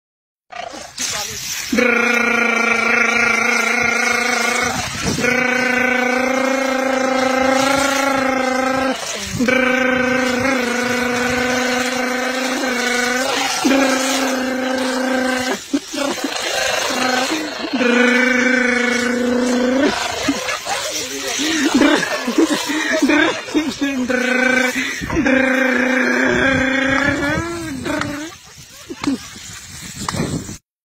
Funny Human Made Grass Cutter Machine | Grass Cutting By Man | Funny Video
Here is the most funniest Video where Grass was Cutting By Man as a machine like. Funny Video. This happens only in India.